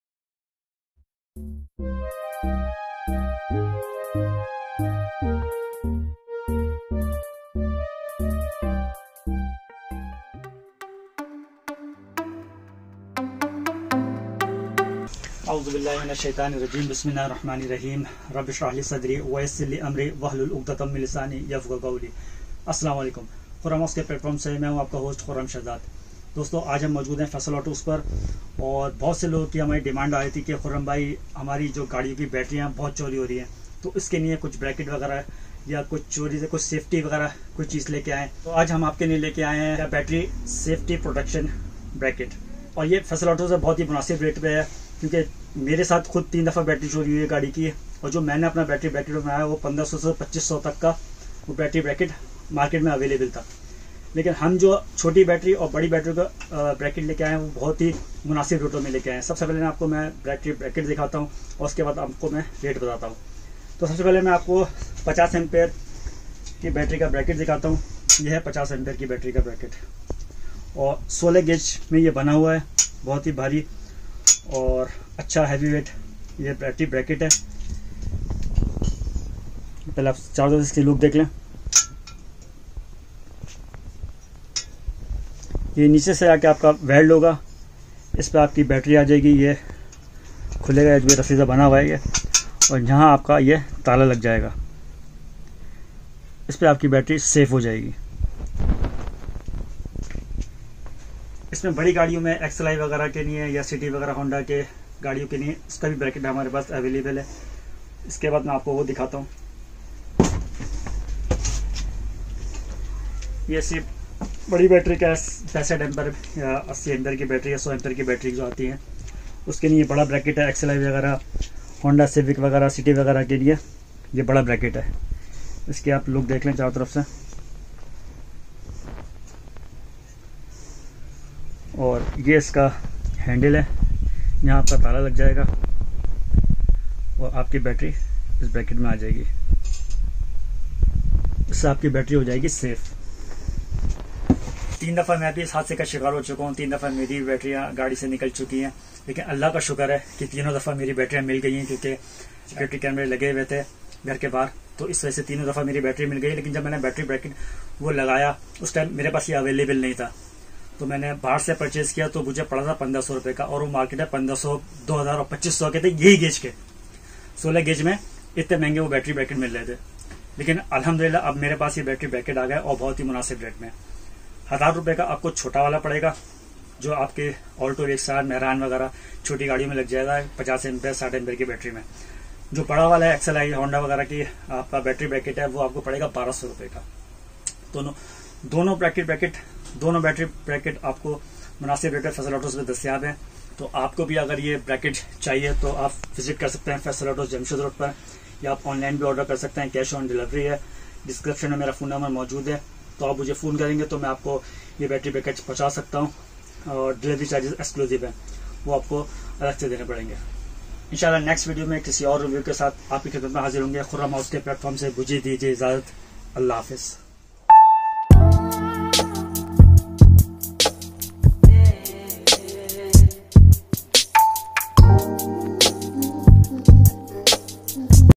car battery theft protection ! car ki battery chori hone se kaiser bachaye
how to prevent your car battery from theft
car battery available at Faisal Autos Jamshed Road.